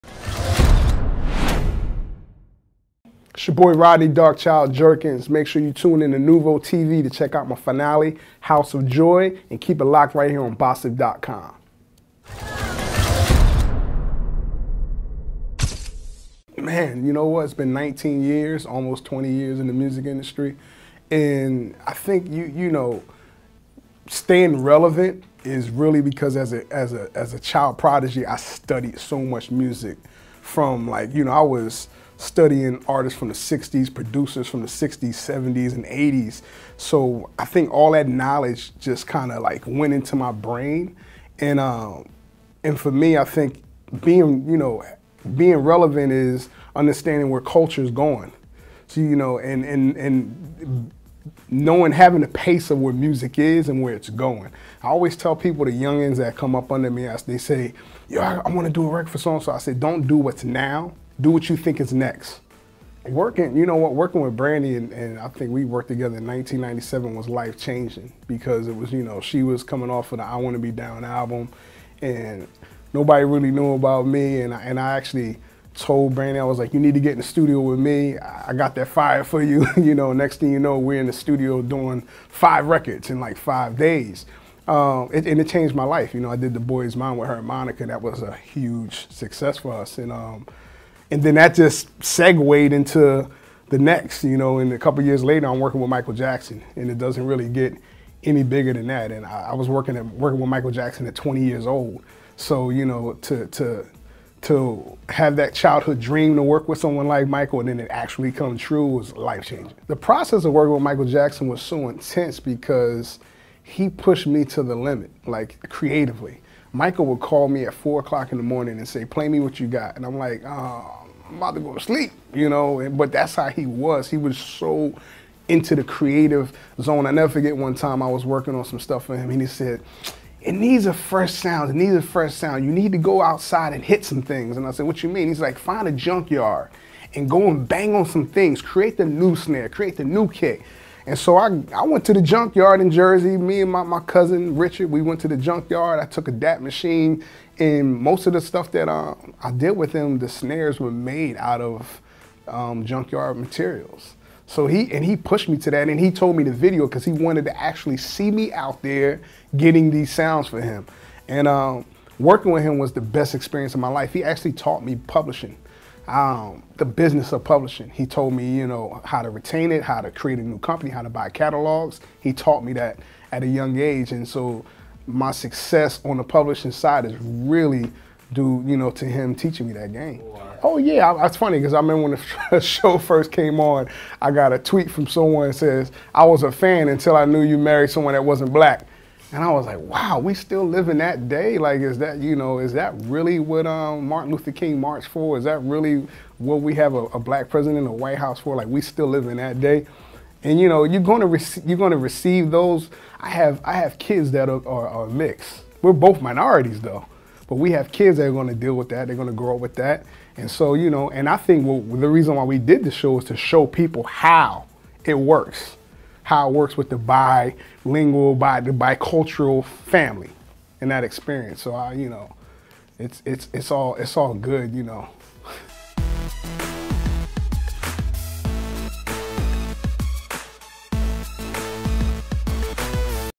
Rodney Jerkins Says Working With Michael Jackson Was The Best Experience Of His Life | BOSSIP
Rodney Jerkins tells all about working with Brandy and Michael Jackson.

Producer Rodney Jerkins talks about longevity in the music business, working with Brandy and Michael Jackson, and being in an interracial relationship and a bi-cultural family.